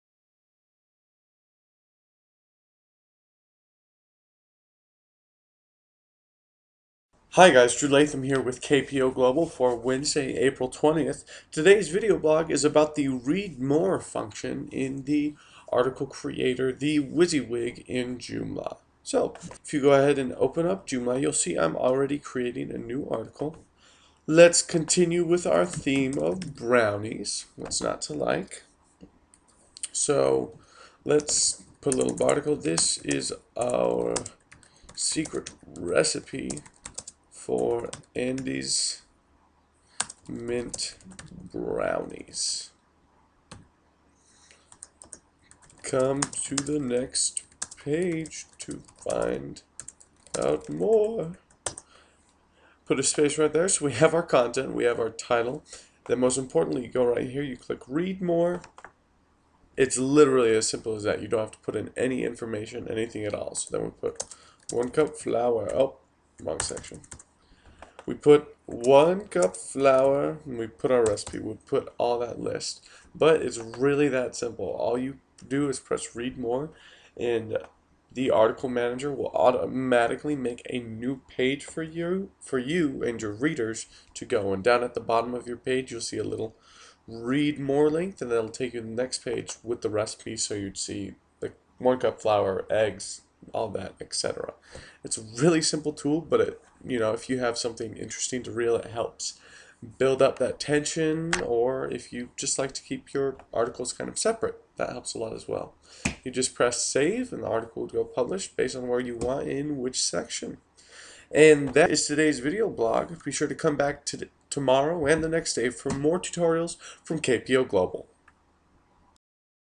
Joomla - Article Page Break
Inset a Page Break into any new or old article.